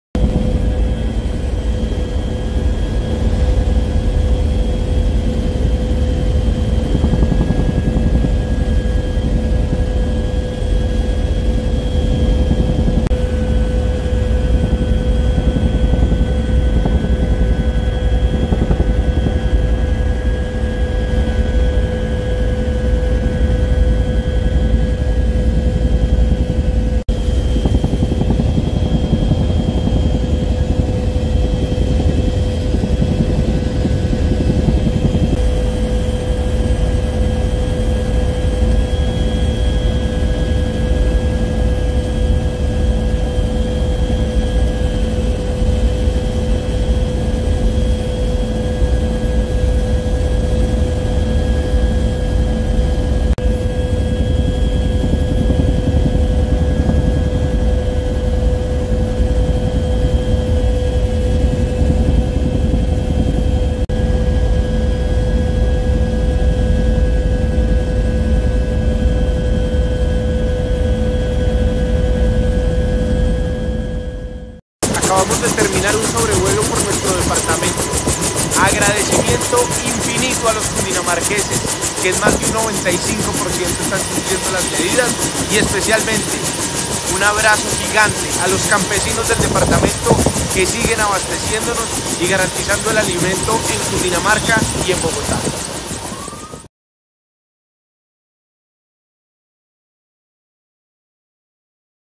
Sobrevuelo por Cundinamarca
Nicolás García Gobernador de Cundinamarca sobrevoló el departamento y dió un parte de tranquilidad pues los cundinamarqueses se han portado muy bien.